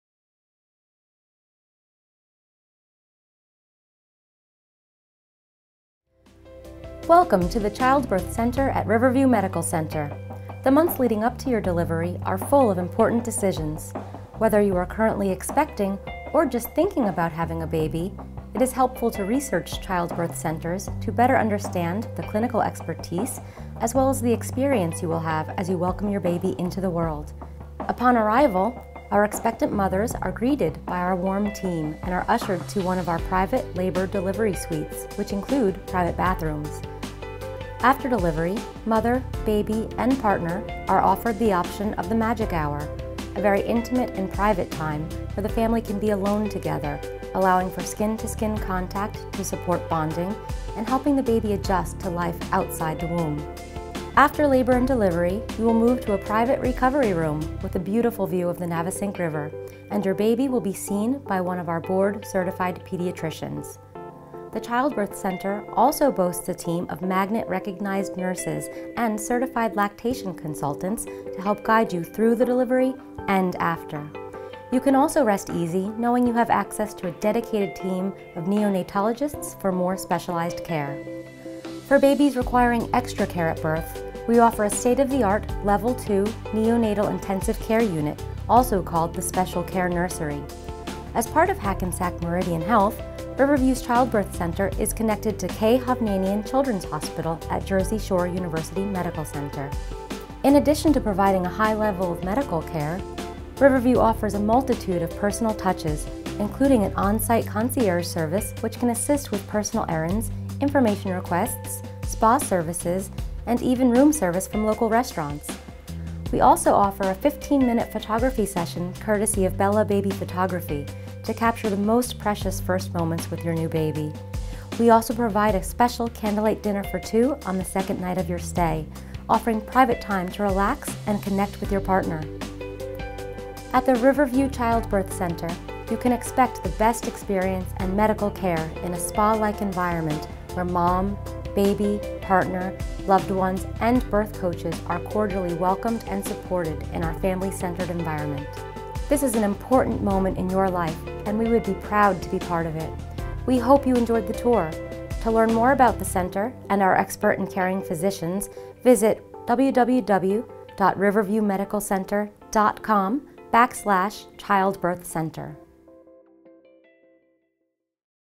Riverview Medical Center Childbirth & Maternity Video Tour
Welcome to the Childbirth and Maternity Center at Riverview Medical Center in Red Bank, N.J., where you can expect the best experience and medical care in a family-centered environment. The months leading up to your delivery are full of important decisions. Whether you are currently expecting or just thinking about having a baby, it is helpful to research childbirth centers to better understand the clinical expertise as well as the experience you will have as you welcome your baby into the world.  This video tour is designed to do just that.

Upon arrival, our expectant mothers are greeted by our warm team and are ushered to one of our private labor delivery suites, which include private bathrooms. After delivery, mother, baby and partner are offered the option of the Magic Hour: a very intimate and private time where the family can be alone together, allowing for skin-to-skin contact to support bonding, and helping the baby adjust to life outside the womb.

After labor and delivery you will move to a private recovery room, with a beautiful view of the Navesink River.  And, your baby will be seen by one of our board certified pediatricians.  The Childbirth Center also boasts a team of magnet-recognized nurses and certified lactation consultants to help guide you through the delivery and after. You can also rest easy knowing you have access to a dedicated team of neonatologists for more specialized care.

For babies requiring extra care at birth, we offer a state-of-the-art, Level 2 neo-natal intensive care unit, also called the Special Care Nursery. As part of Hackensack Meridian Health, Riverview’s Childbirth Center is connected to K. Hovnanian Children’s Hospital at Jersey Shore University Medical Center..

In addition to providing a high level of medical care, Riverview offers a multitude of personal touches, including an onsite concierge service, which can assist with personal errands, information requests, spa services, and even room service from local restaurants. We also offer a 15-minute photography session, courtesy of Bella Baby Photography, to capture the most precious first moments with your new baby.  We also provide a special candlelight dinner for two on the second night of your stay, offering private time to relax and connect with your partner.

At the Riverview Childbirth Center, you can expect the best experience and medical care in a spa-like environment where mom, baby, partner, loved ones, and birth coaches are cordially welcomed and supported in our family-centered environment.

This is an important moment in your life, and we would be proud to be a part of it. WE hope you enjoyed the tour. To learn more about our Center and our expert and caring physicians